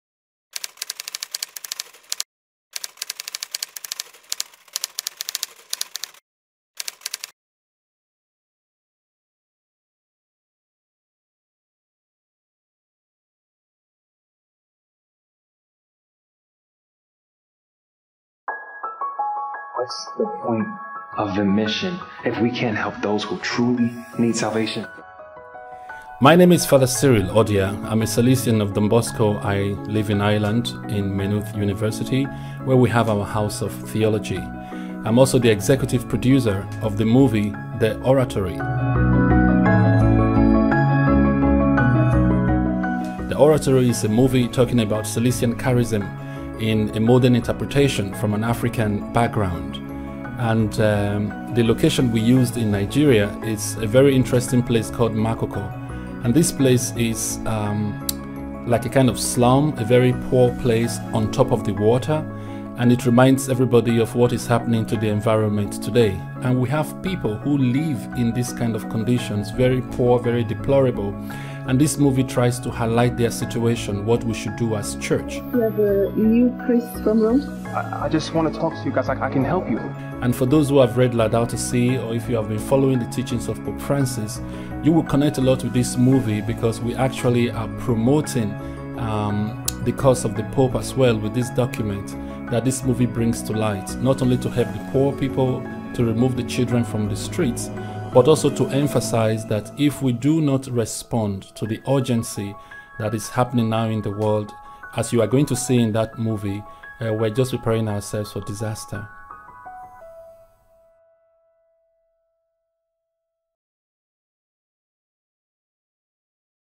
"O Oratório": das estradas de Lagos, em um filme, o grito dos pobres e da terra
Estreado em alguns cinemas da Europa, o longa-metragem produzido e dirigido pelo diretor de cinema nigeriano Obi Emelonye conta a história de um grupo de garotos de Lagos cuja sobrevivência, não raramente, está ligada ao crime. Por trás de um cenário sombrio, marcado pela degradação e pobreza, está a luz de Laudato si' na reinterpretação moderna da figura de São João Bosco e no desejo dos jovens de contribuir para a causa do meio ambiente.